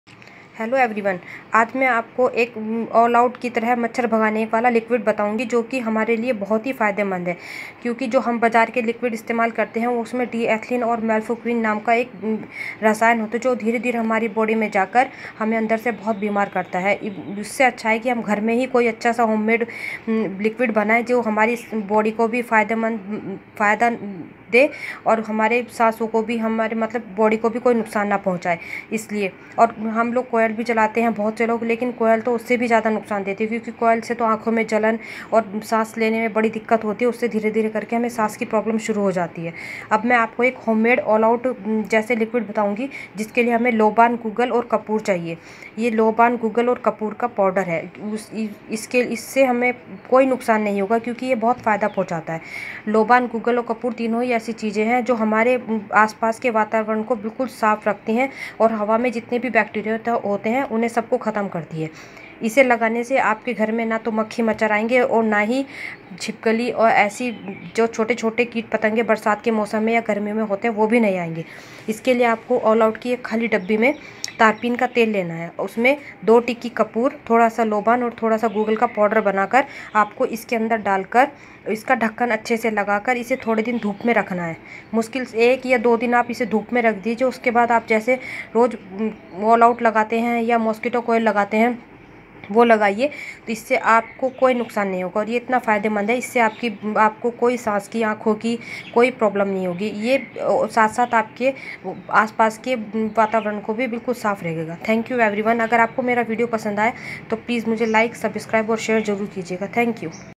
Homemade mosquito killer no saidefact for our body
Mosquito killer // loban and gugal bouth hi comman  hai aap ko kisi bhi kirina ka store sa mil gaye ge. dono lo barabar lana hai .full tea spoon.